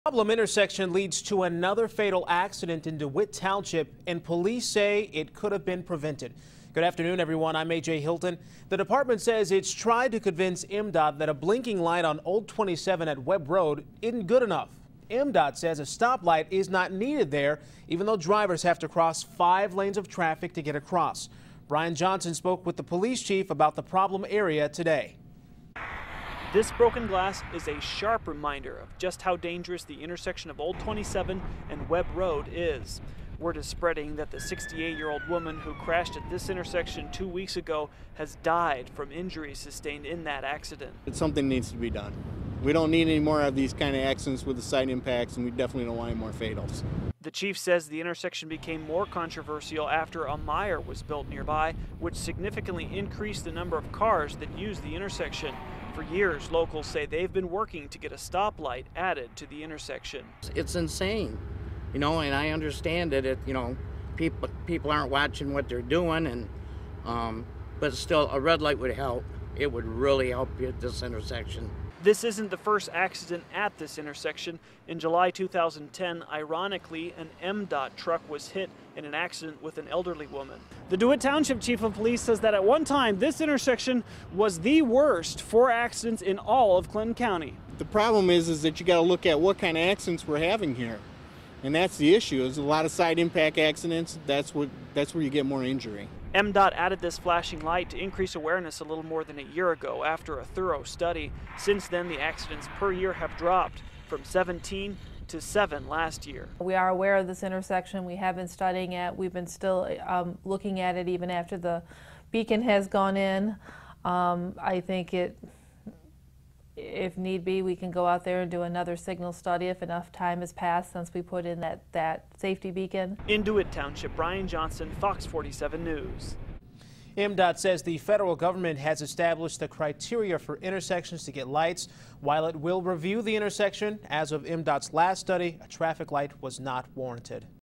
Fatal Accident Has Police Chief Worried About Intersection
A fatal accident at an intersection in DeWitt Township has police outraged. That's because they've tried to convince MDOT in the past that there needs to be a stop light on Old 27 at Webb Road. MDOT has said there isn't a need for a stop light there-